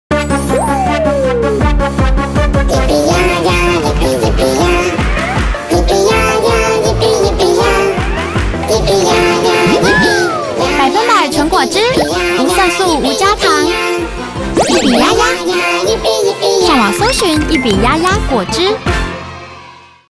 品牌形象 小編推薦 iOiO YiBiYaYa 100%果汁系列
-100%純果汁，保留水果原始風味
-綜合莓果汁，富含12種莓果類果汁精華
-綜合蔬果汁「十蔬十果」，富含多達20種蔬菜水果的營養素
-適合不愛吃蔬果的嬰幼兒
-無色素、無香料、人工甘味劑
-無添加糖，保留水果本身的甜味
-大瓶蓋設計，安全避免孩童誤食
-開蓋即食，隨身包設計，方便攜帶